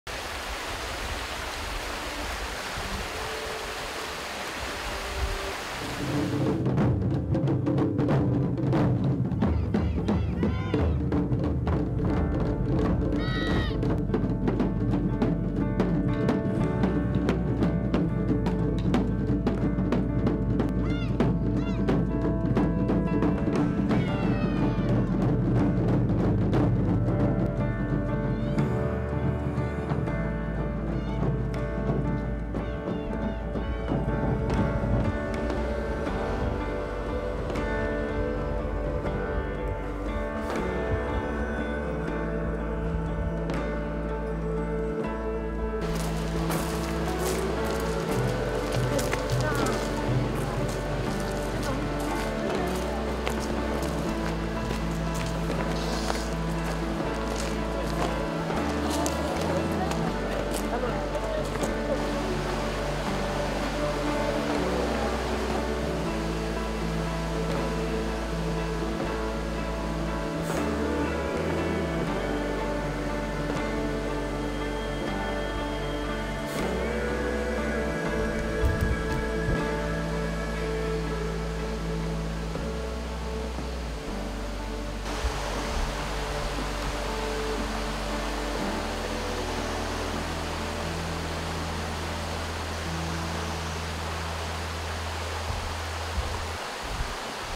【津別ニュース】第14回クリンソウまつり開催
【津別ニュース】2019年6月22日23日北海道津別町上里で毎年恒例のクリンソウまつりが行われた。
今回で14回目となる。

道東テレビはみなさまのご支援で運営しております。
JIMBAオンラインサロン　月額1,080円